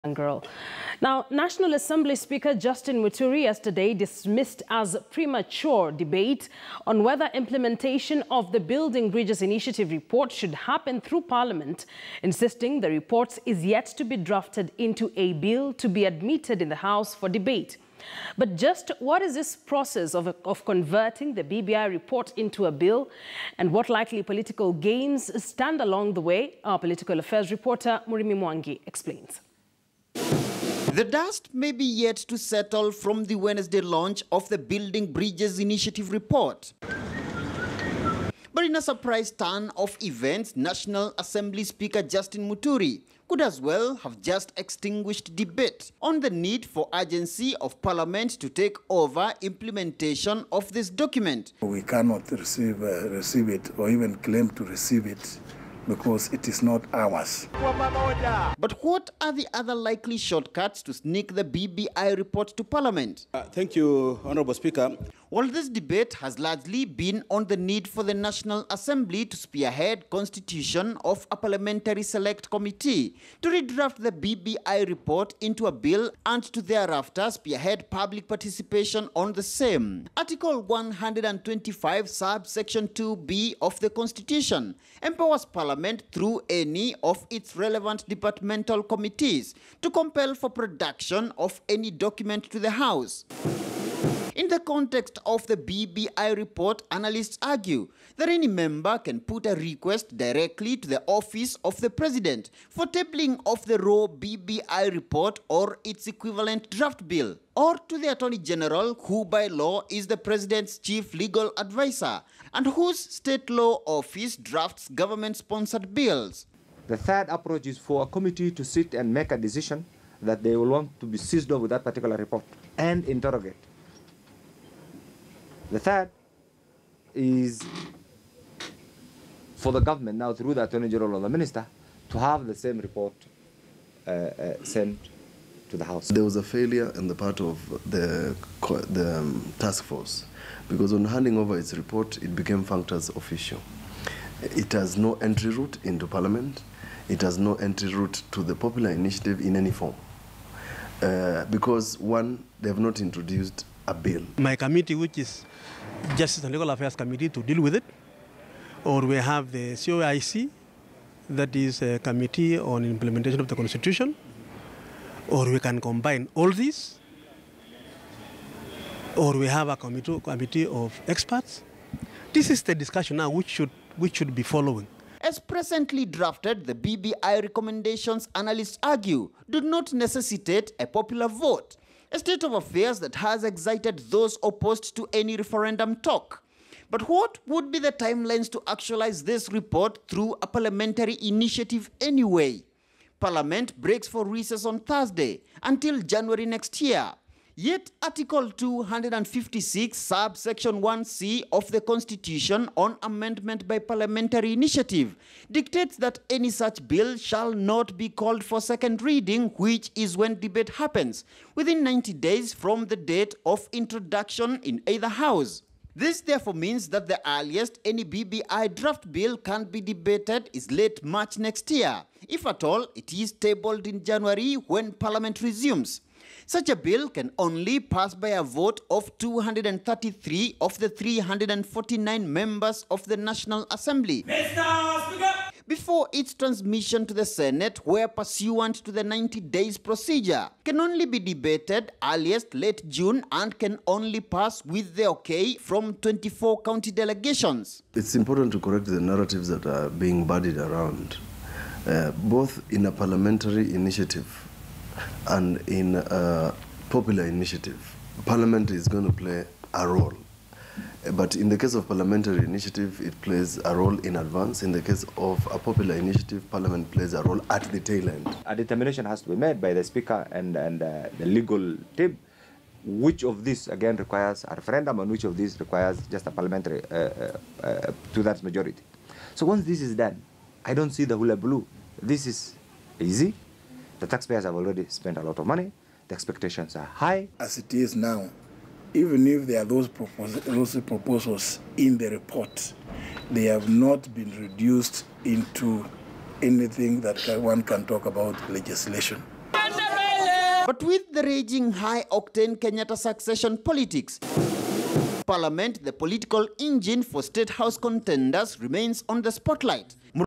Justin Muturi: We cannot receive the BBI report because it doesn't belong to us
National assembly speaker Justin Muturi yesterday dismissed as premature debate on whether implementation of the building bridges initiative report should happen through parliament, insisting the report is yet to be drafted into a bill to be admitted in the house for debate. But just what is this process of converting the BBI report into a bill, and what likely political games stand along the way?